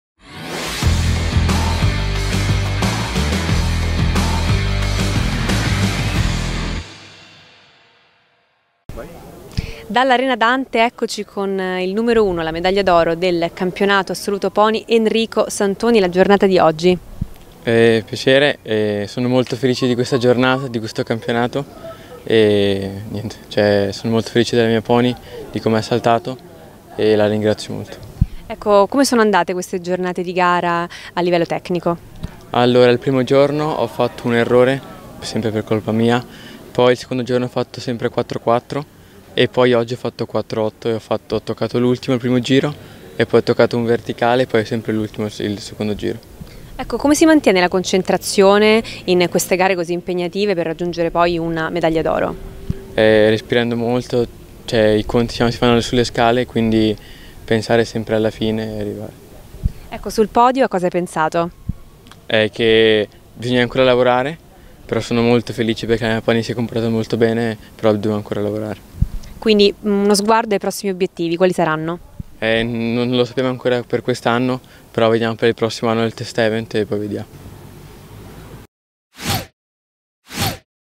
Intervista a Enrico Santoni - Campionati Italiani Giovanili Selleria Equipe
Intervista a Enrico Santoni vincitore dell'Assoluto Pony ai Campionati Italiani Giovanili Selleria Equipe